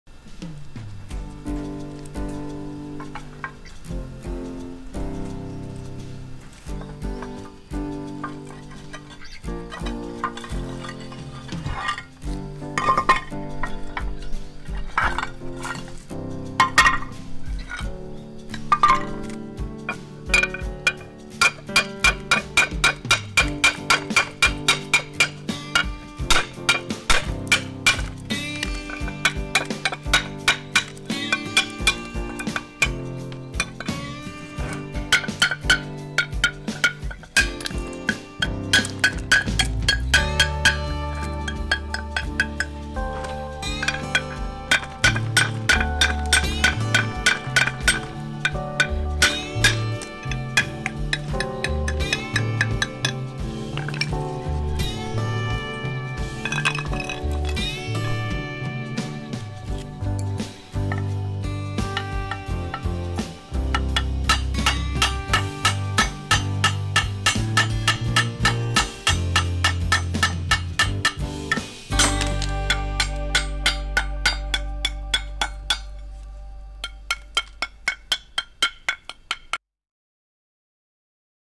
ステイホーム1
ステイホームということで、ただひたすら鉄平石を割る動画。

It's stay home week, so I immersed myself in breaking stones to stay strong and positive.